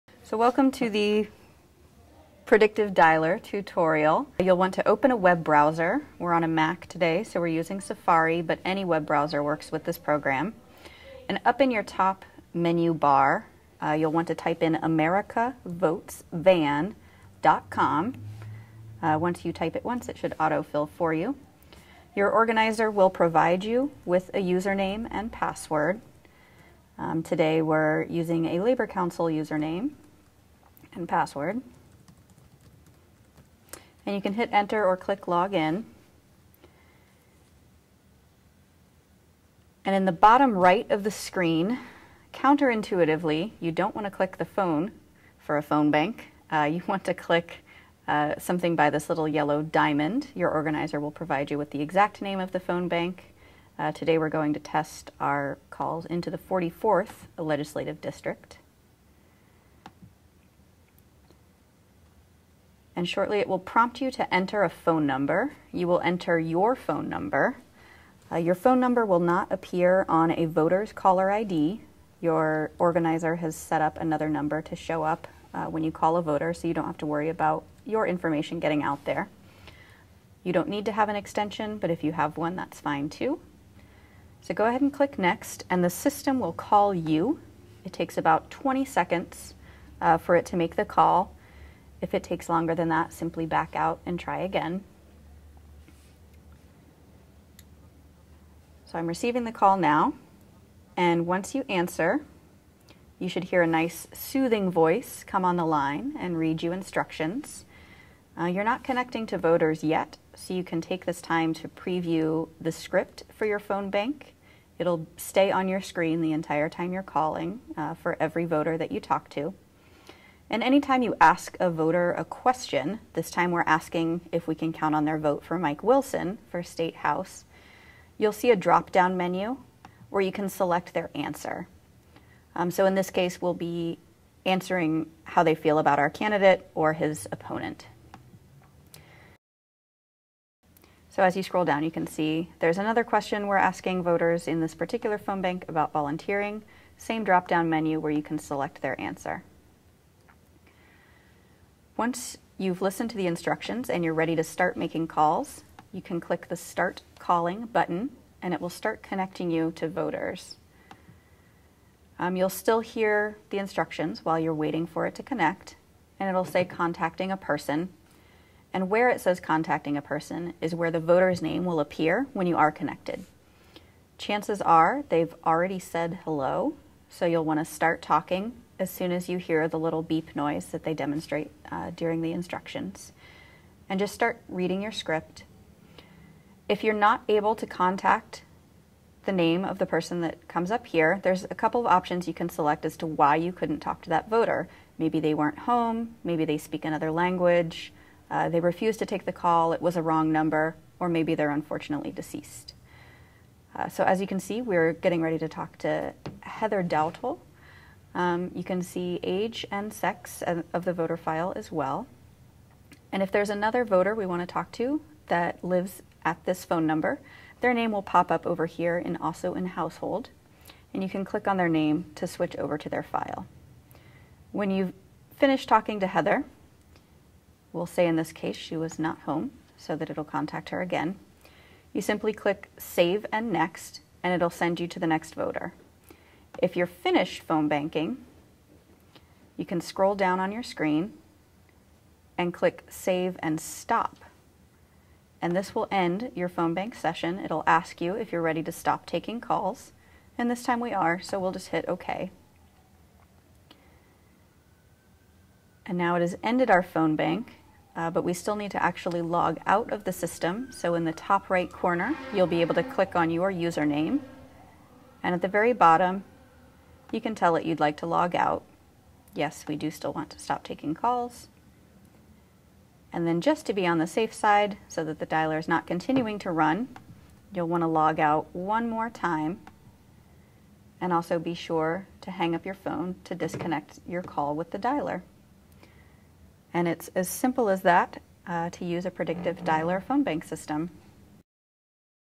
Van Predictive Dialer Tutorial
Learn how to remotely log into the VAN to make calls from your phone. This does not log you in, you will have to get a login and password from your organizer  - but it is a step by step guide to get you on the system and ready to make your calls.